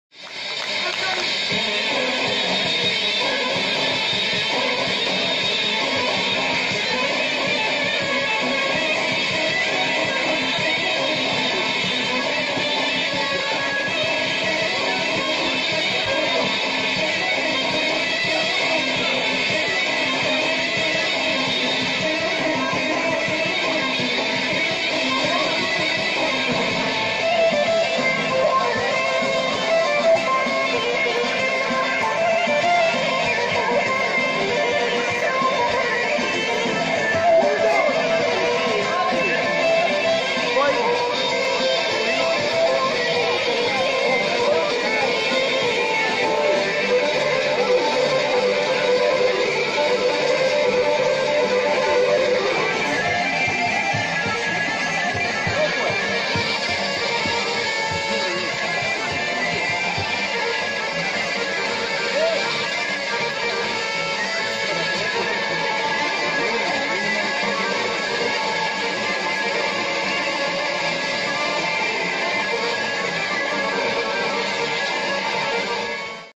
emFOLKLOR - Nastupzaraju Zabrdje Vitez 2018
Razigrani i neumorni - članovi BZK Preporod Vitez i nastup za raju na platou PD Zabrđe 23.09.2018